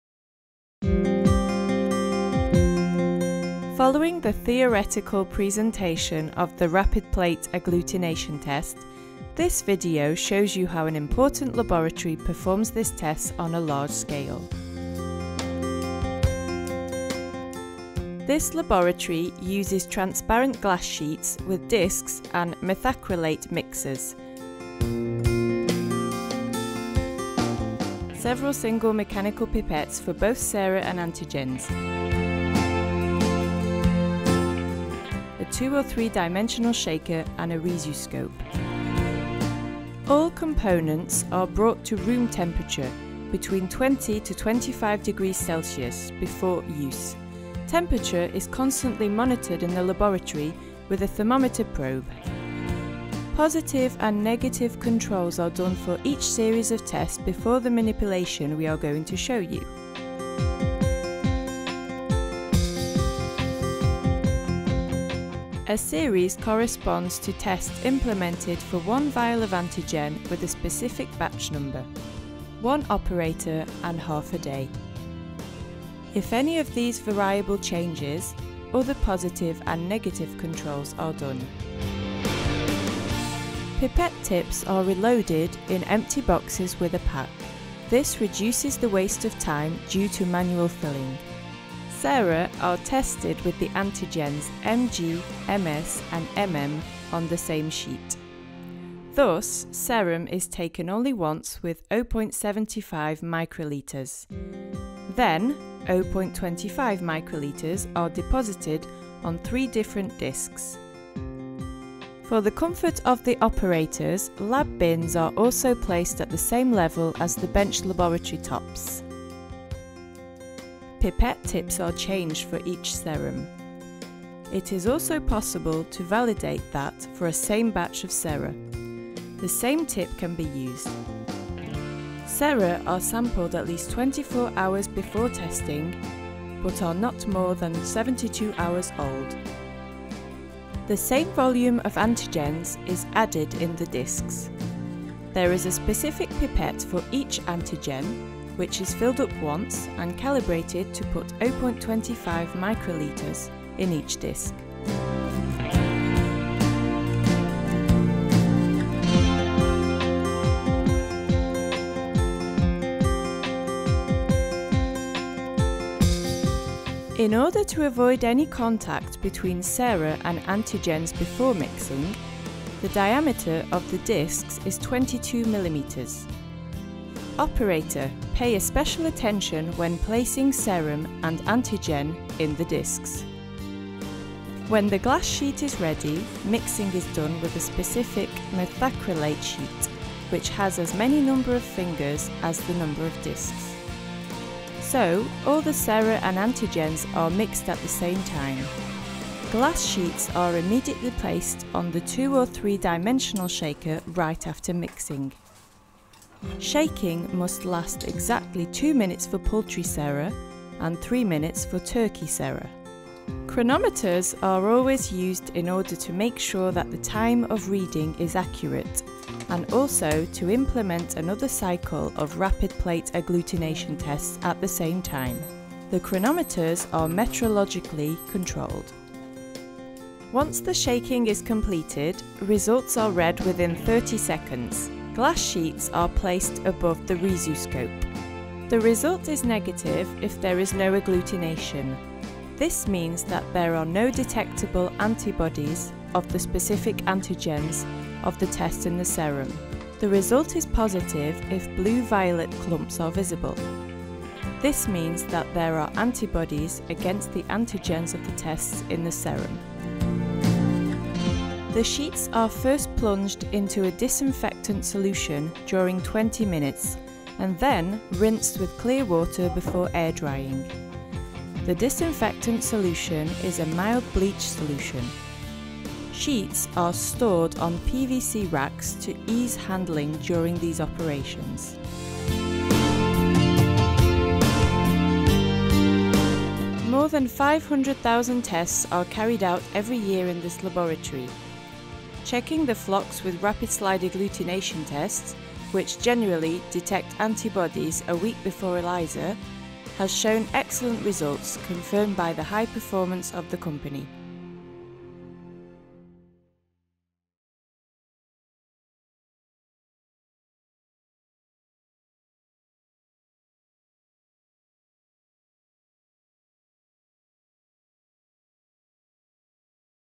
Rapid Slide Agglutination test Biovac and Soleil
Biovac Soleil Rapid plate agglutination test in an veterinary analysis laboratory in France